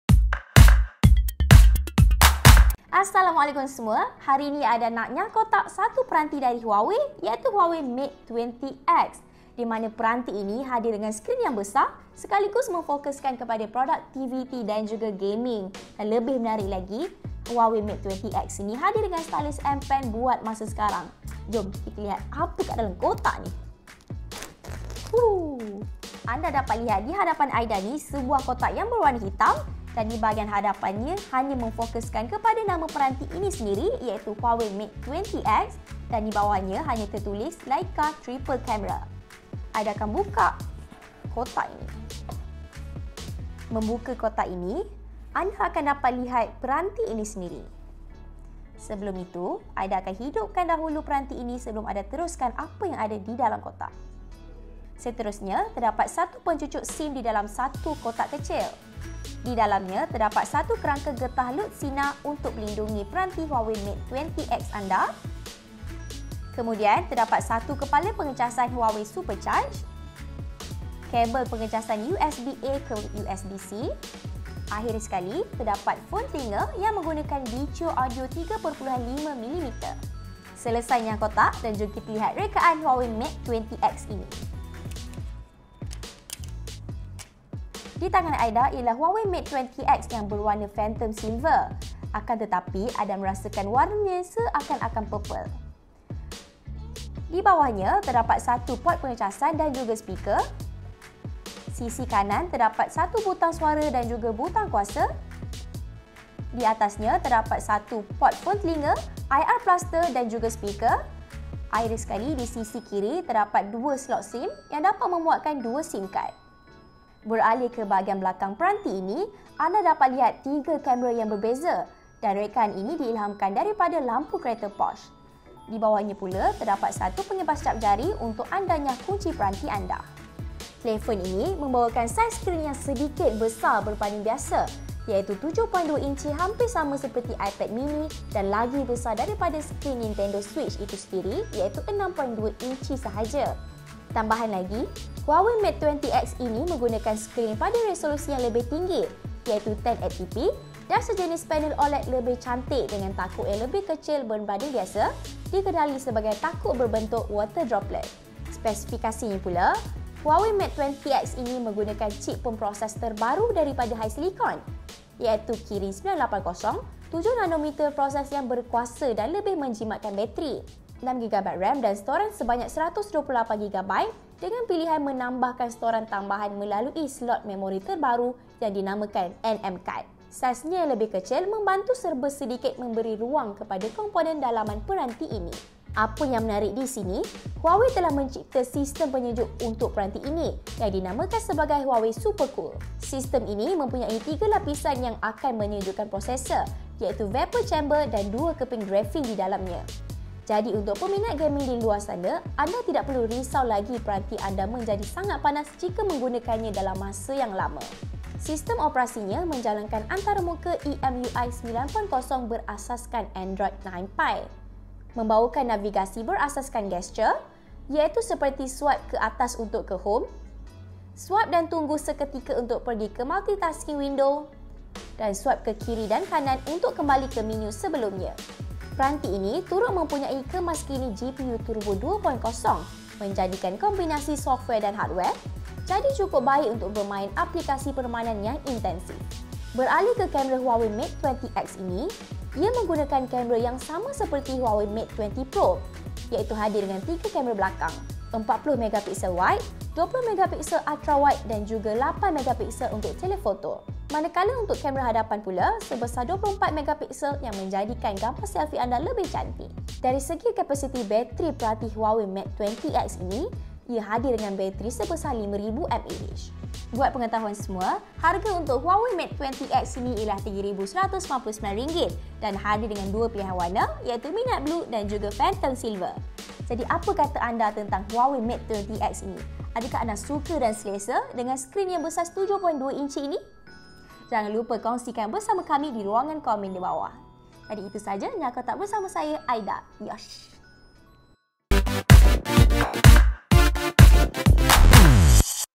Nyah-kotak Huawei Mate 20X - Telefon Gaming Huawei?
Huawei Mate 20X merupakan salah satu peranti yang telah dilancarkan oleh Huawei dibawah siri Mate 20. Ia hadir dengan skrin 7.2-inci, dijana cip Kirin 980, dan dilengkapi sokongan penyejukan Vapor untuk menyokong masa penggunaan yang lama. Di Malaysia, Huawei Mate 20X dijual pada harga RM3199.